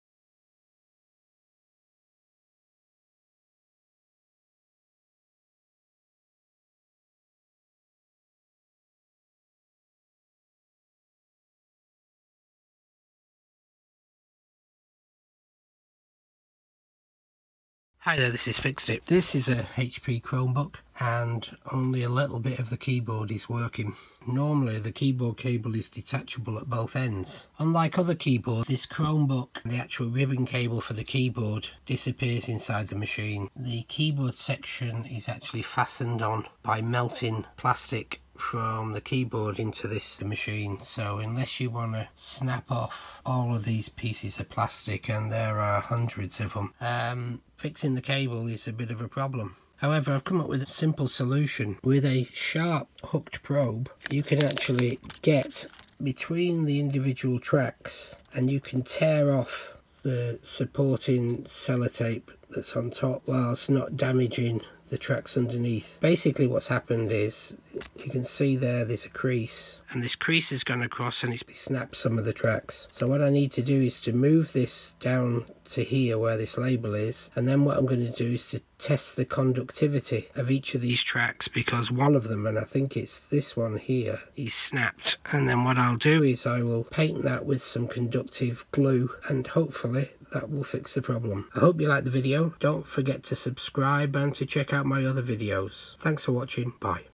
How To Fix A Faulty Google Chromebook Keyboard - Part 1
If this video helps you can buy @fixed1t a coffee Thx. The item I used to successfully make the fix is available here

Keyboards Regularly Become Defective And Need Fixing Or The Keyboard Replacing

But The Price Of A Replacement Keyboard For A HP Google Chromebook Is Over £99 I'm Not Paying That.

So I've Chosen To Fix It Myself In This Multi-Part Video

HP Chromebook Keyboard Fix - Part 1

Malcolm aka @fixed1t